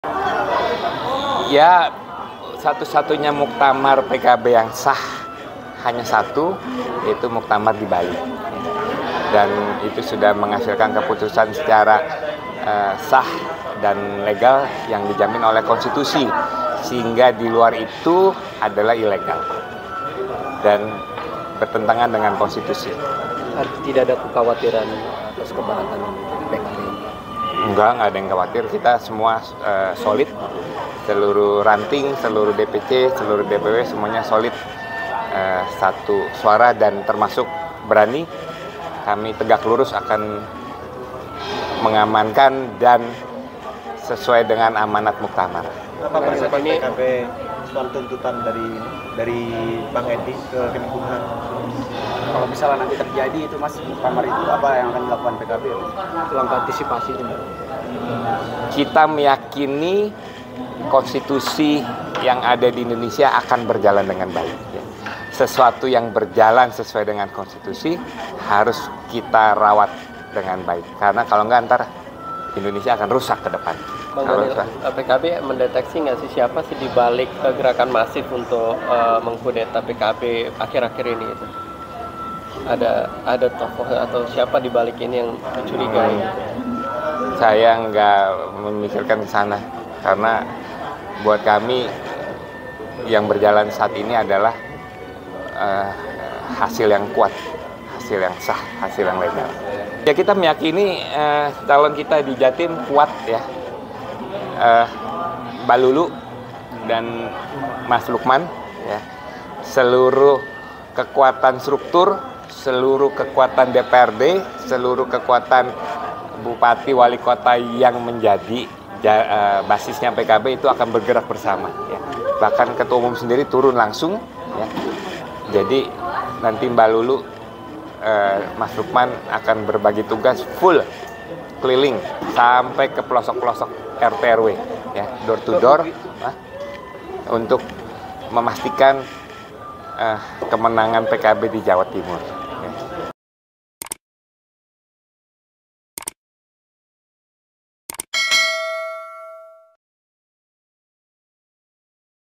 Diadukan ke Kemenkumham, PKB Sebut Muktamar di Bali Sah Secara Konstitusi
Ketua Dewan Pimpinan Pusat (DPP) Partai Kebangkitan Bangsa (PKB) Daniel Johan menegaskan muktamar PKB di Bali sah secara konstitusi. Muktamar di Bali bahkan membuat kader kuat dan kokoh.

Daniel mengungkapkan hal itu setelah menghadiri acara Mukernas I Berani, Jumat (30/8/2024), di Swiss-Bellin Hotel Kemayoran, Jakarta Pusat.

Adapun Daniel mengungkapkan hal itu setelah eks Sekretaris Jenderal PKB Lukman Edy menyebut muktamar PKB di Bali sesat dan cacat prosedur. Lukman juga meminta Kemenkumham tidak mengesahkan struktur pengurus hasil Muktamar Bali.

Simak informasinya dalam video berikut ini.

Video Jurnalis: Dimas Nanda Krisna
Penulis Naskah: Dimas Nanda Krisna
Video Editor: Dimas Nanda Krisna
Produser: Nursita Sari